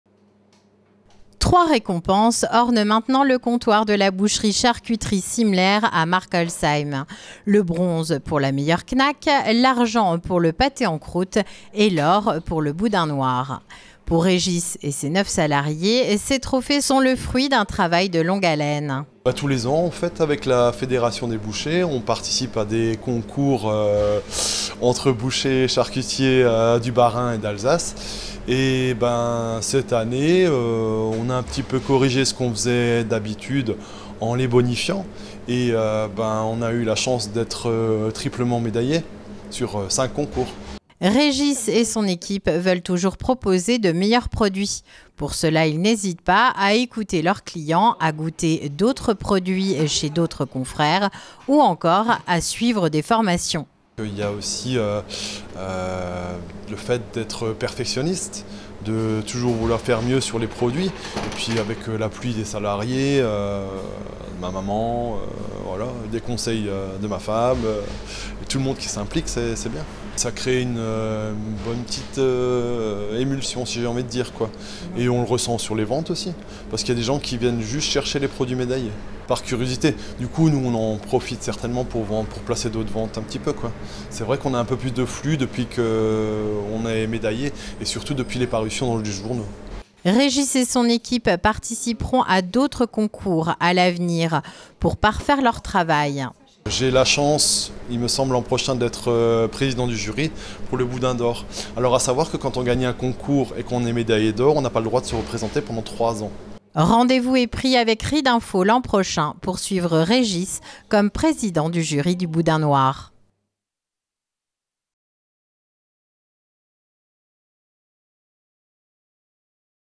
Les médailles de la boucherie charcuterie Simler et Fils
La boucherie charcuterie Simler a obtenu 3 médailles pour son travail : le bronze pour la knack, l'argent pour le pâté en croute, et l'or pour le boudin noir.